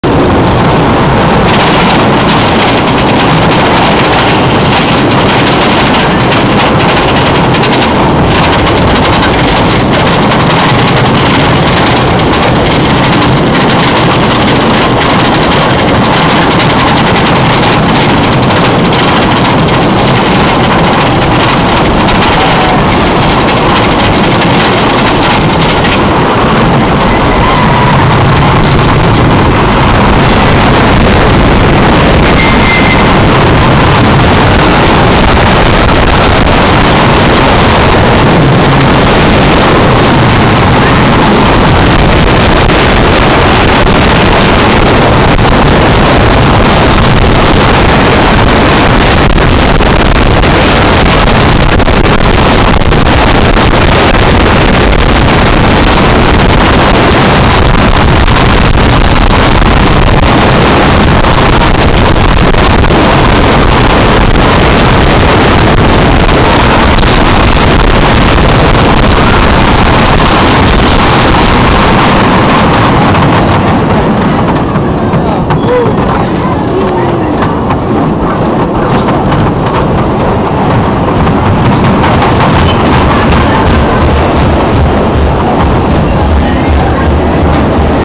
Joris en de Draak - onride (water) - Efteling (Sneak Preview, 29 juni 2010)
onride van de waterbaan van joris en de draak in de efteling